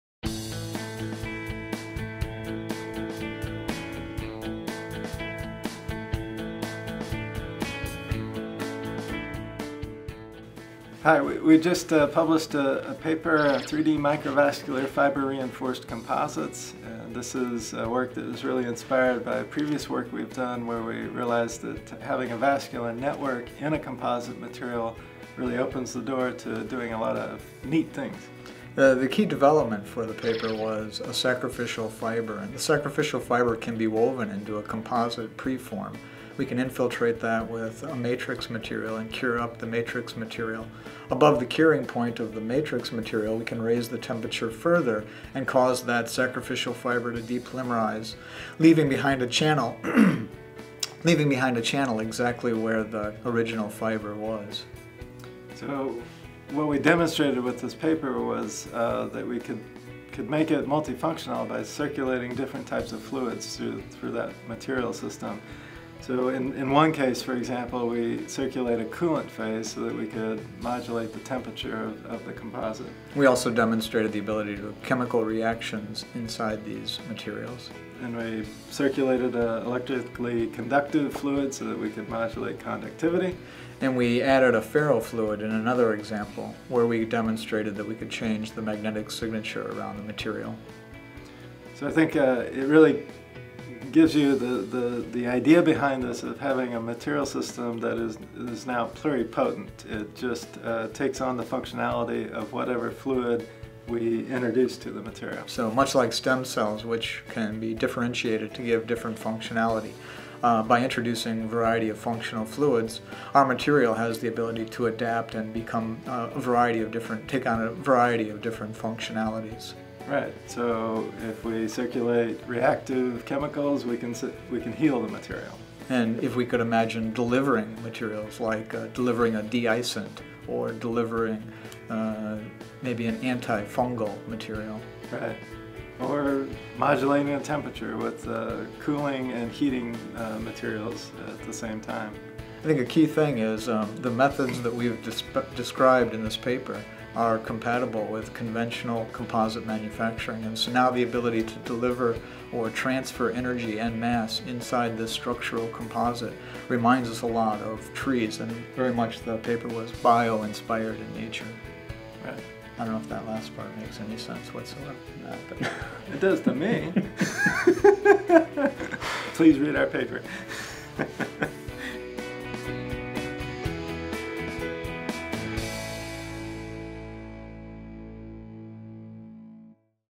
Video Abstract: Fiber Reinforced Composites
In this video abstract, Professors Jeffrey Moore and Scott White talk us through their newest publication, "Three-Dimensional Microvascular Fiber-Reinforced Composites".

A method for fabricating microvascular networks in fiber-reinforced composites is presented. The method relies on sacrificial fibers woven into fiber preforms that, when removed by depolymerization and volatilization, create 3D microvascular networks inside the composite material. By circulation of functional liquids in the resulting channels, a diverse set of new functionality is demonstrated. Simplicity, robustness, scalability, and reliance on readily available components make this method compatible with composite manufacturing methods.